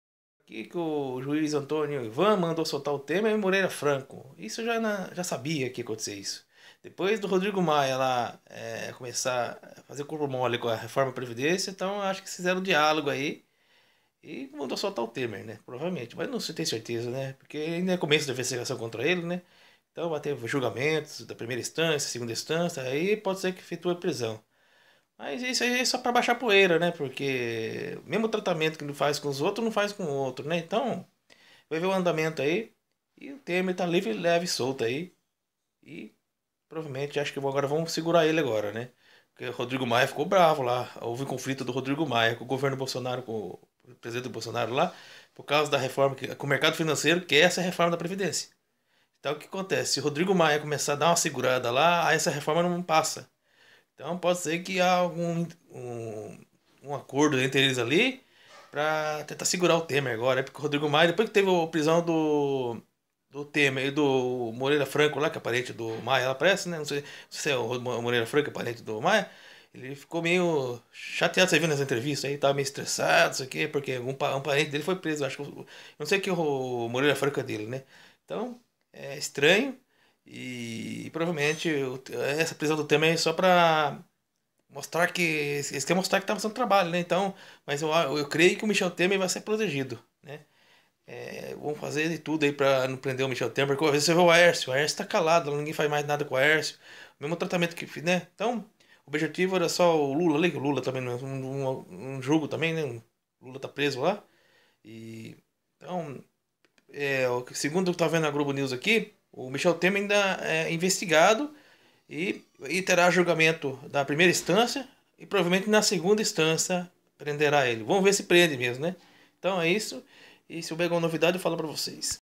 Justiça manda solta ex presidente Temer Moreira Franco Maia que influenciou judiciário vejam...
Justiça manda solta Michel Temer Moreira Franco Parente do Rodrigo Maia depois atrito Maia Bolsonaro nas rede sociais prisão Parente dele provavelmente Maia peça chave principalmente das reformas previdência interesses do mercado financeiro .